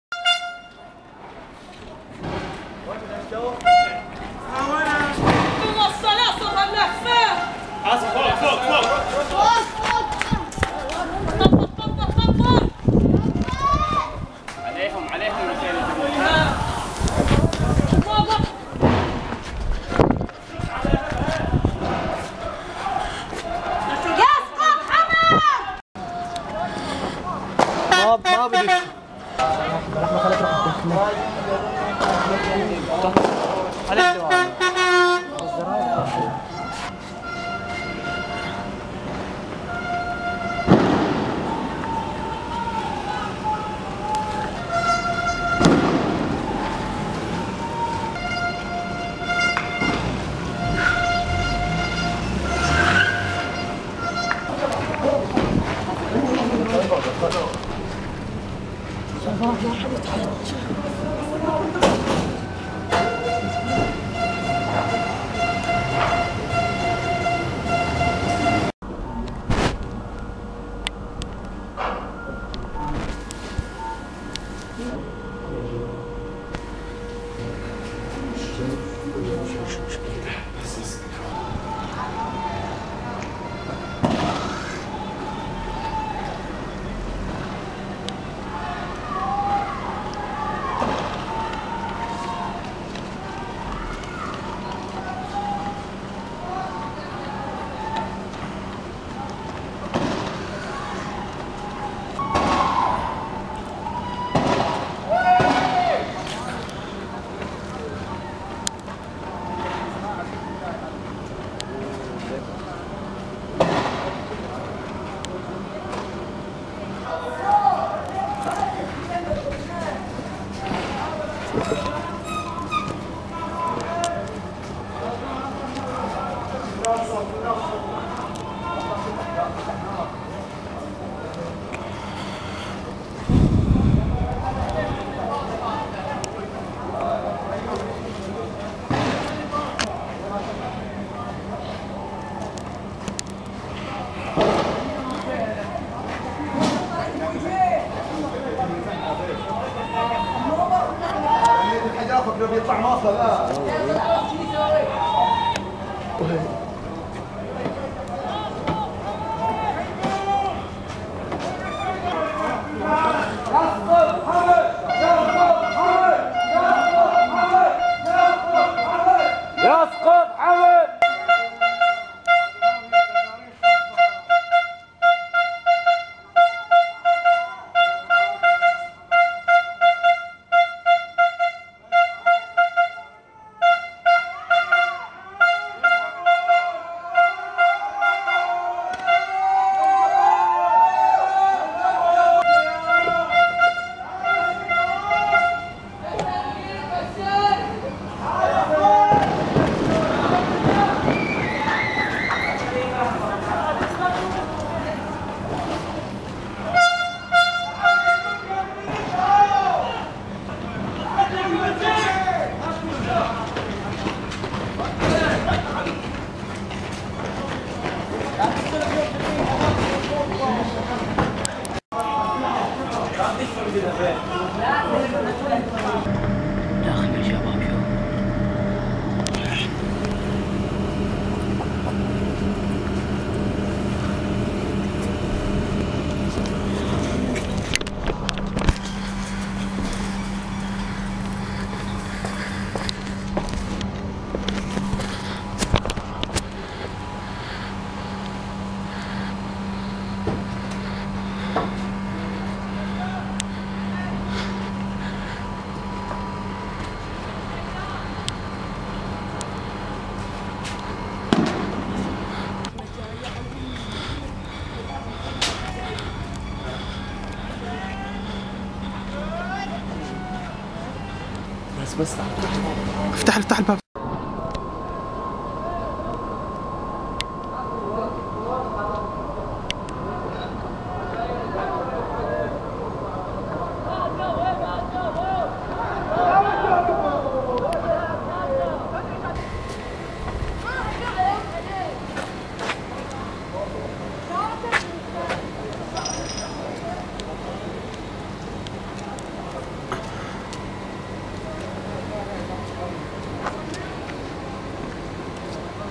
مواجهات أبوصيبع و العقاب الجماعي عليها 30 8 2011
همجية قوات الأمن في الأستخدام المفرط للغازات السامة على القرى تسببت في الكثير من الأختناقات للأهالي بالقرى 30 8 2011